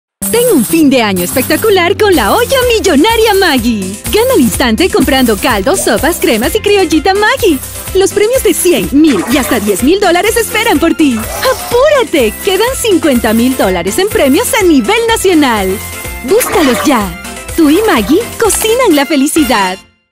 Olla Millonaria MAGGI®
Vuelve la Olla Millonaria de MAGGI® apresúrate, aún hay $50.000 en premios ¡Búscalos ya!